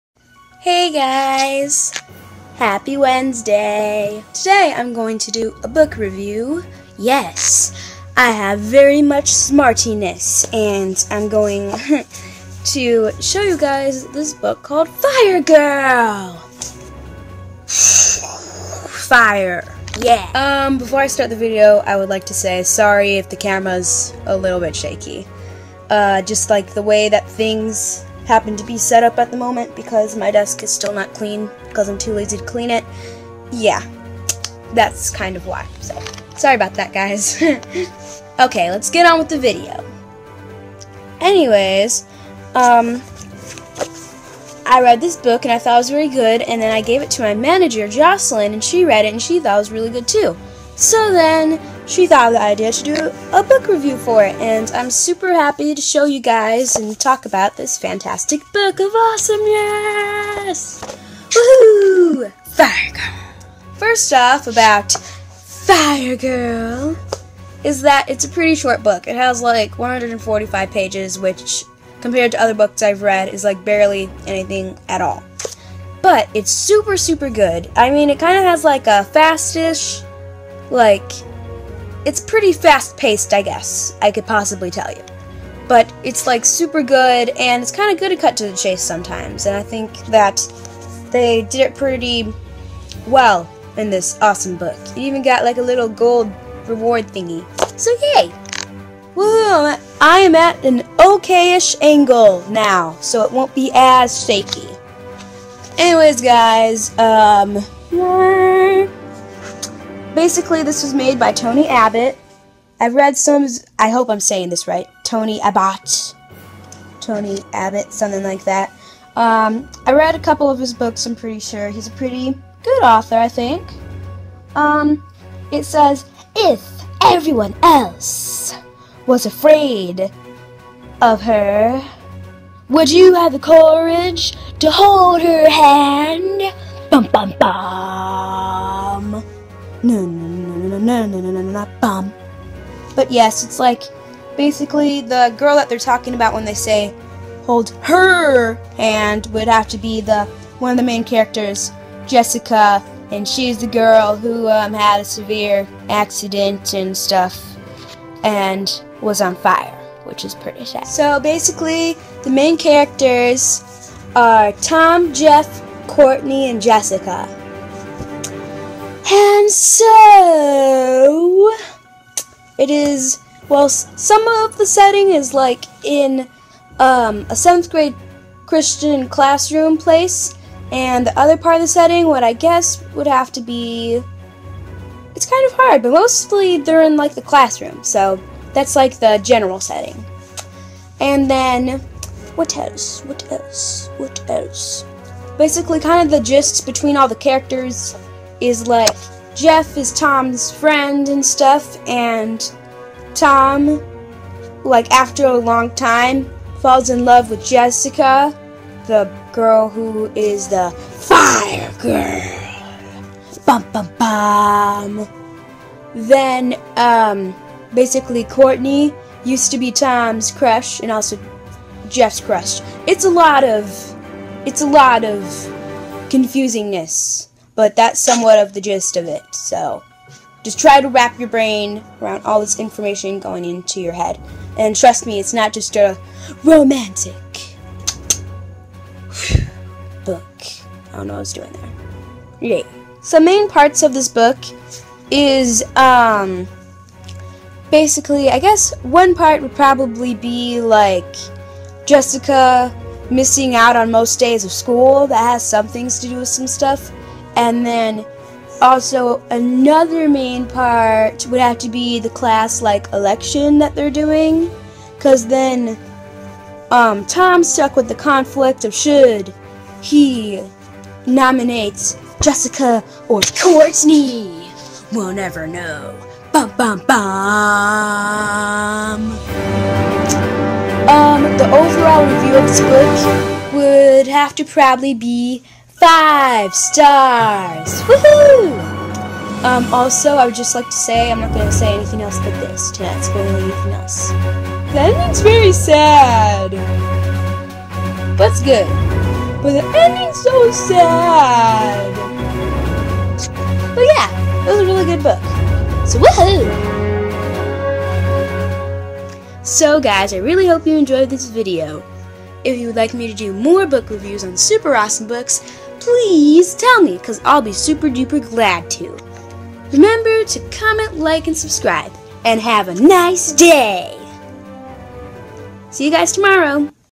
"Fire Girl" Book Review
Fire Girl is a pretty cool book made by Tony Abbott. Me and my friend really enjoy it so we hope you could to! Have a nice day!